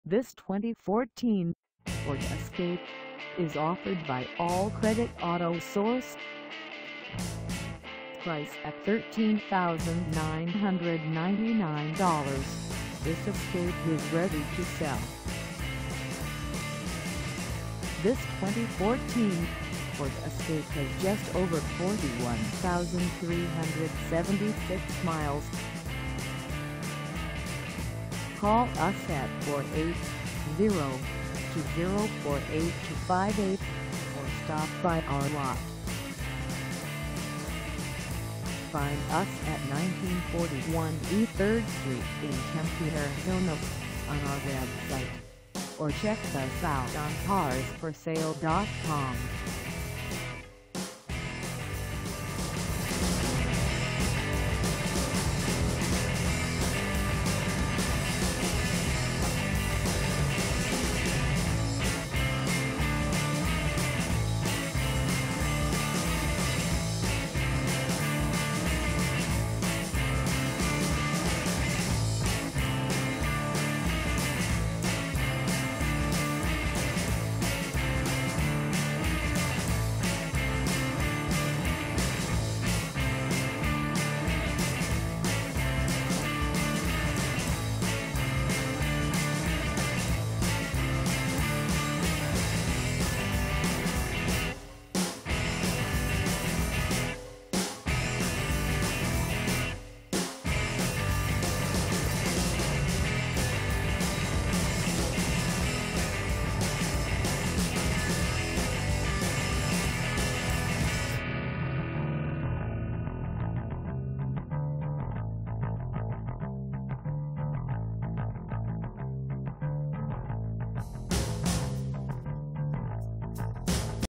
2014 Ford Escape S 4dr SUV for sale in Tempe, AZ 85281 at Ba
This 2014 Ford Escape S 4dr SUV is for sale in Tempe, AZ 85281 at Bad Credit Auto Source.

Engine: 2.5L
Transmission: Automatic 6-Speed
Color: White
Mileage: 41376
Contact All Credit Auto Source 1941 E. 3rd Street Tempe, AZ 85281 to test drive this 2014 Ford Escape S 4dr SUV today.